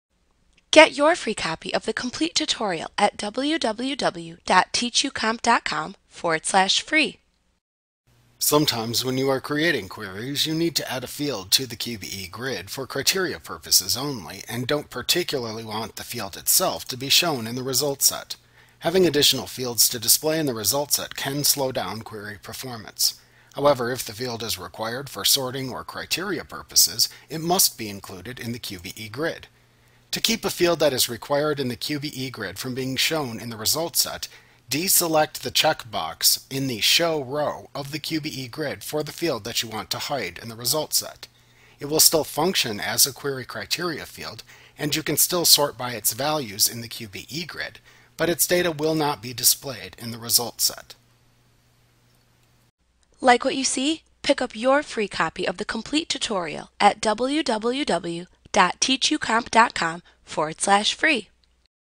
Access 2016 Tutorial Hiding Fields in a Result Set Microsoft Training
Learn how to hide fields in a result set in Microsoft Access - the most comprehensive Access tutorial available. Visit us today!